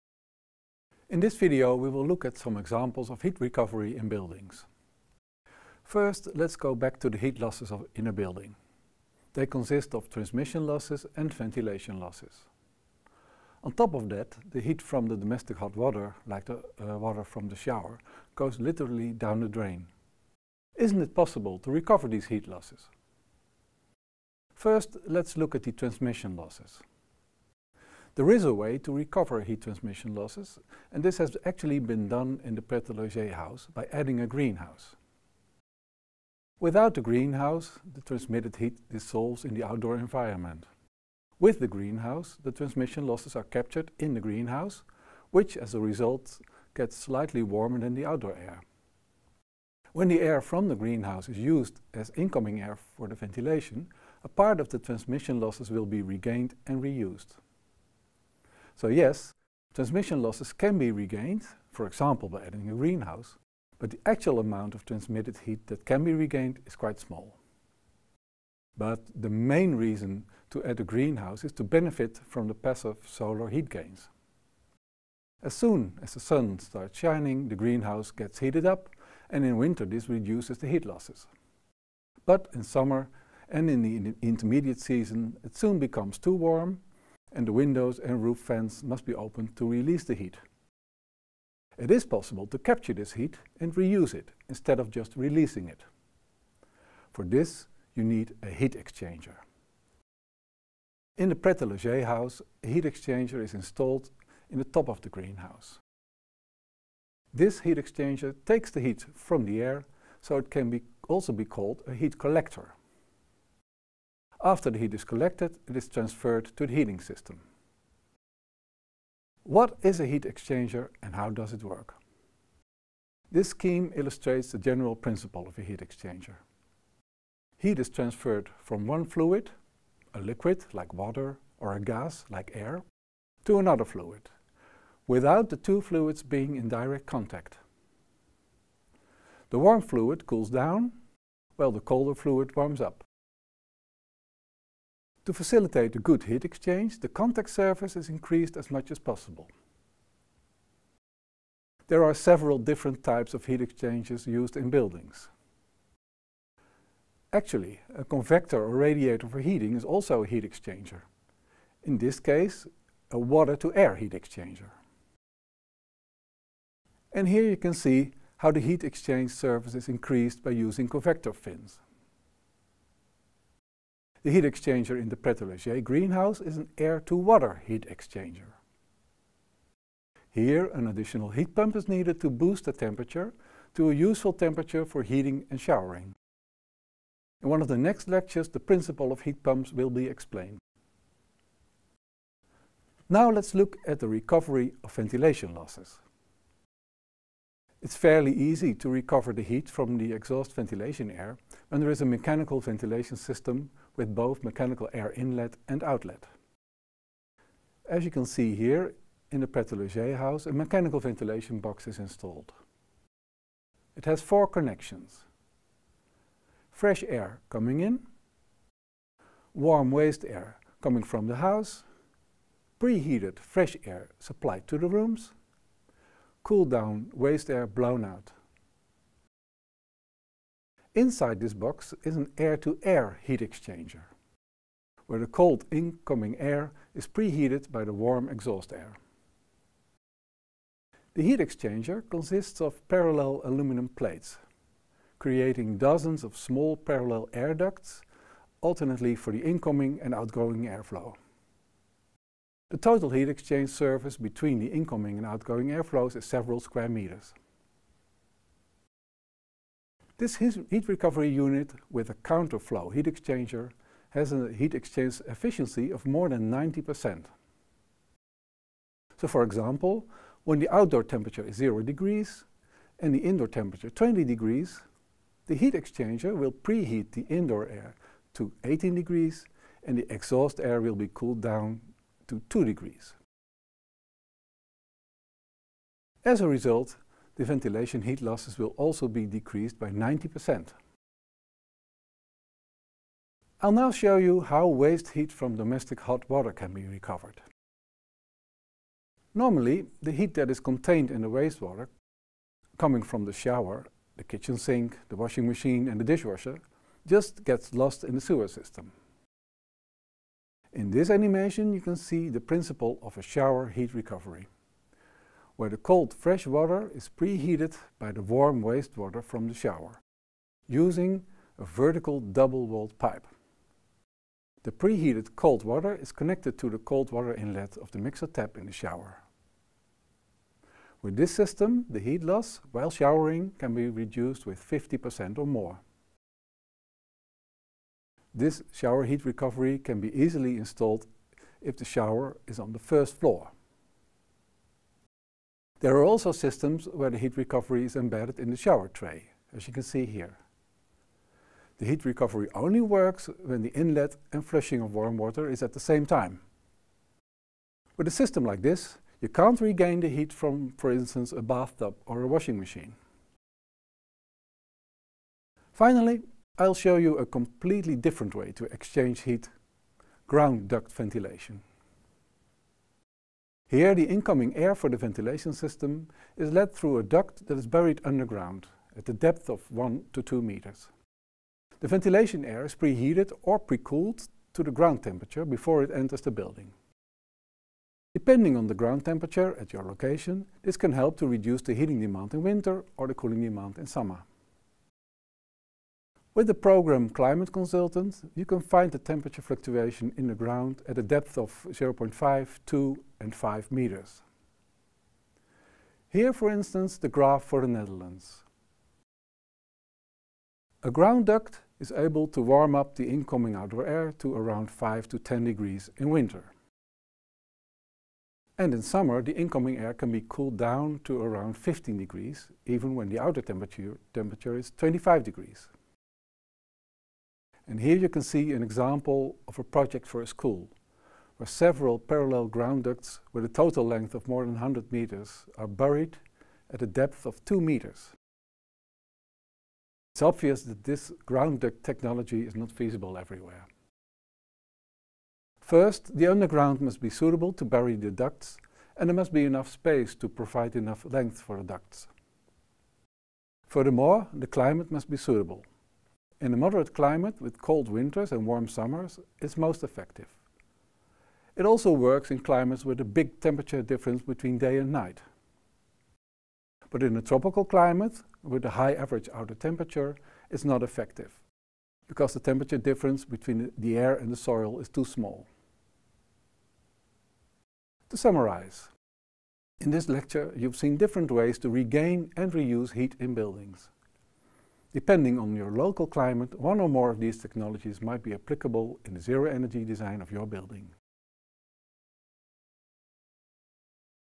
4.2.1 Heat Recovery
In this video Eric will show you different ways to regain and reuse heat in buildings.